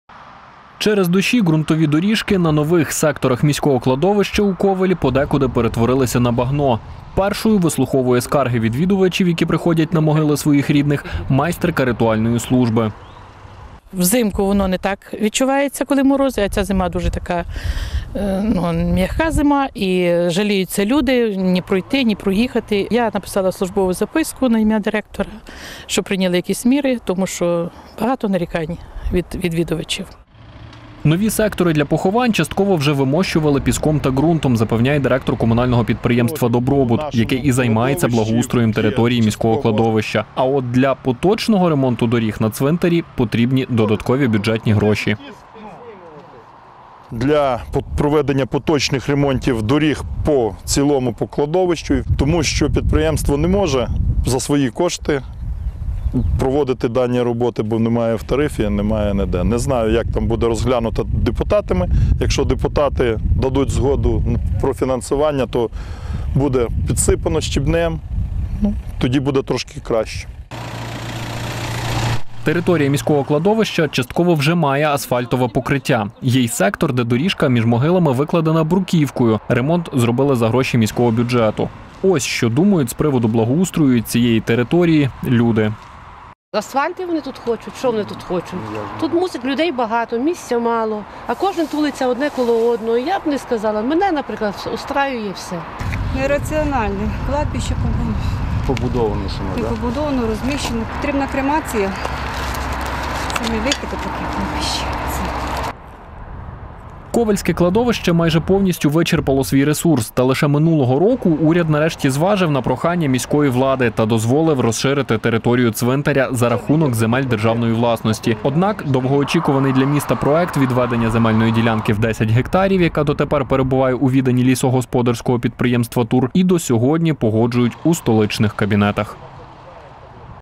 У Ковелі - проблема із міським кладовищем
Через дощі ґрунтові доріжки на нових секторах міського кладовища у Ковелі подекуди перетворилися на багно.

Нові сектори для поховань частково вже вимощували піском та ґрунтом, - запевняє Ігор Михалевич - директор комунального підприємства "Добробут", яке і займається благоустроєм території міського кладовища. А от для поточного ремонту доріг на цвинтарі потрібні додаткові бюджетні кошти.

Територія міського кладовища частково вже має асфальтове покриття. Є й сектор, де доріжка між могилами викладена бруківкою (ремонт зроблили за гроші міського бюджету).

Ковельське кладовище майже повністю вичерпало свій ресурс. Та лише минулого року уряд, нарешті, зважив на прохання міської влади та дозволив розширити територію цвинтаря за рахунок земель державної власності. Однак, довгоочікуваний для міста проект відведення земельної ділянки в 10 гектарів, яка дотепер перебуває у віданні лісогосподарського підприємства “Тур”, і до сьогодні погоджують у столичних кабінетах.